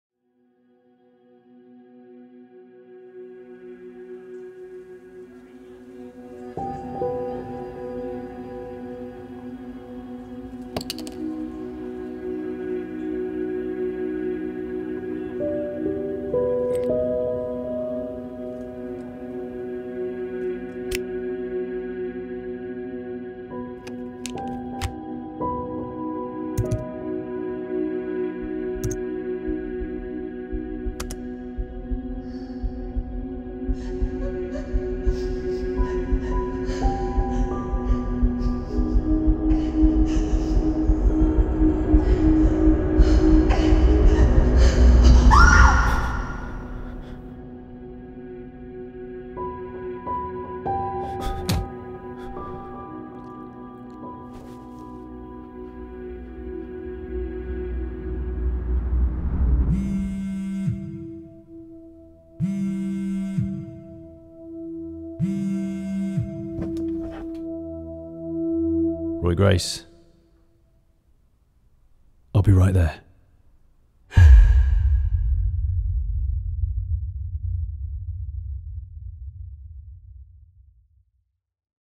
Looking Good Dead Trailer 2021
The thrilling Roy Grace series from multi-million No.1 best-selling author Peter James returns with the world premiere stage production of Looking Good Dead, starring award-winning actor and EastEnders icon, Adam Woodyatt.

No good deed goes unpunished…hours after finding a discarded USB memory stick, Tom Bryce, (Adam Woodyatt) inadvertently becomes a witness to a vicious murder. Reporting the crime to the police has disastrous consequences, placing him and his family in grave danger. When Detective Superintendent Roy Grace becomes involved, he has his own demons to contend with, while he tries to crack the case in time to save the Bryce family’s lives.

Don't miss your chance to see the world premiere tour of Peter James' gripping new play.